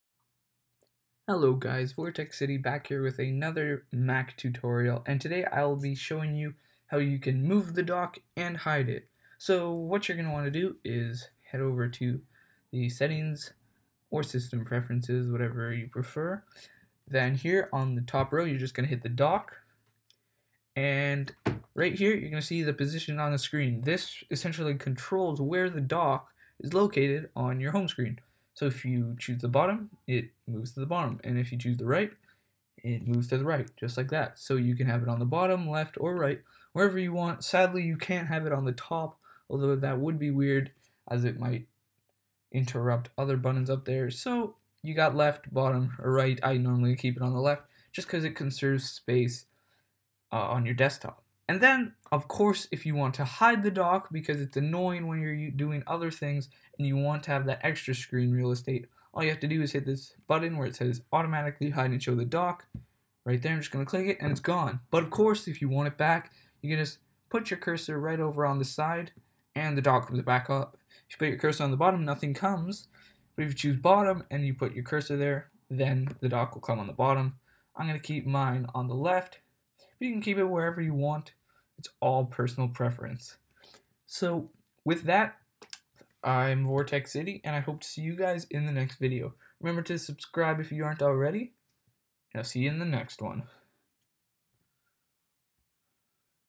How to Hide and Move Dock on Mac (2024)
Today I will be showing you how to hide and move the dock on your Mac device. This would be important in you want to conserve screen space or if you simply do not enjoy the bar on the bottom